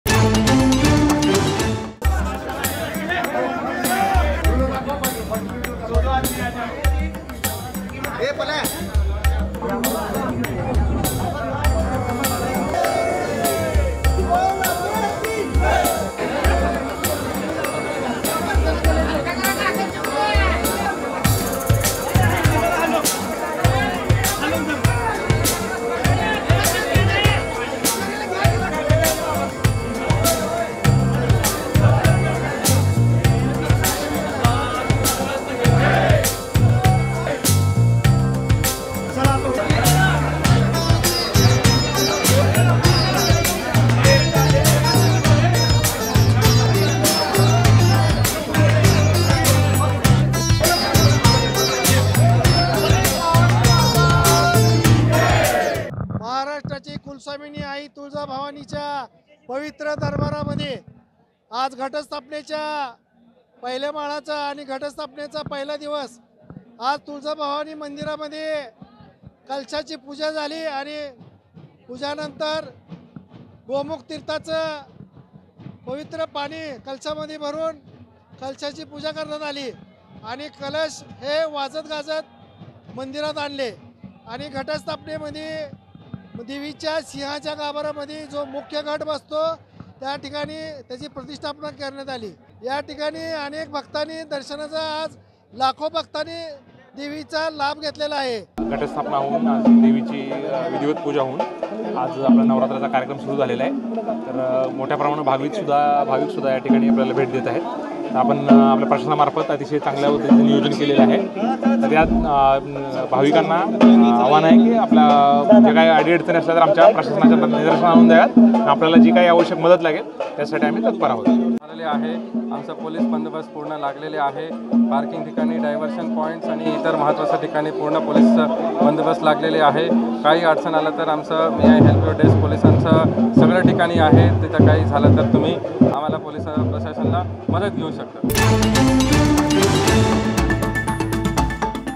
Tuljabhavani: नवरात्रोत्सवाला आजपासून सुरुवात, तुळजाभवानीच्या मूळ मूर्तीची सिंहासनावर प्रतिष्ठापना
नवरात्रोत्सवाला आजपासून सुरुवात, तुळजाभवानीच्या मूळ मूर्तीची सिंहासनावर प्रतिष्ठापना